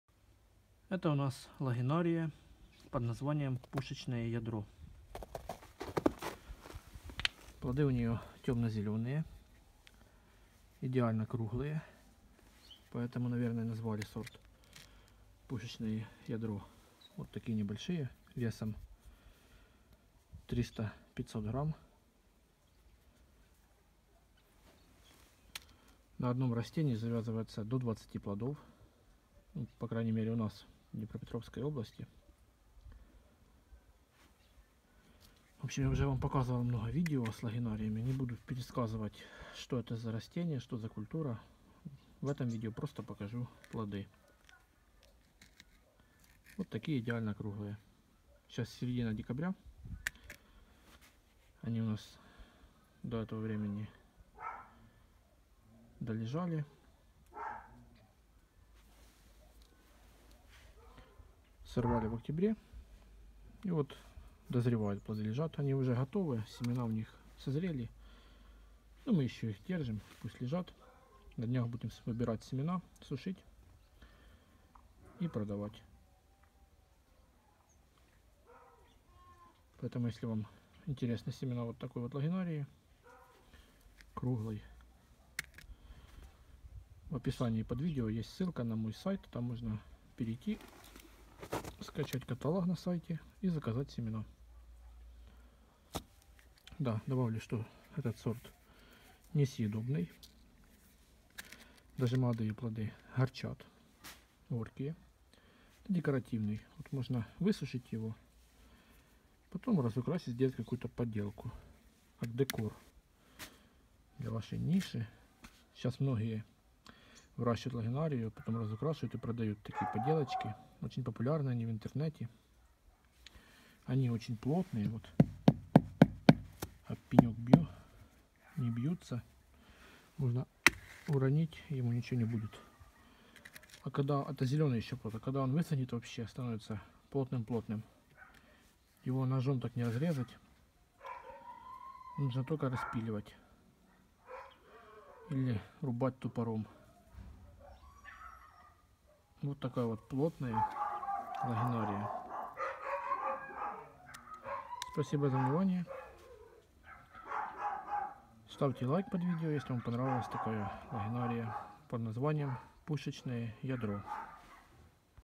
Лагенария ПУШЕЧНОЕ ЯДРО. Lagenaria Canon Ball La
Лагенария ПУШЕЧНОЕ ЯДРО—плоды идеально круглые, темно-зеленого цвета, весом до 1 кг,  очень много на кусте, красота, надо только увидеть.
Lagenaria Canon Ball La - The fruits of this variety are absolutely round and are ideal for an exceptional decoration. Diameter: 12 - 14 cm.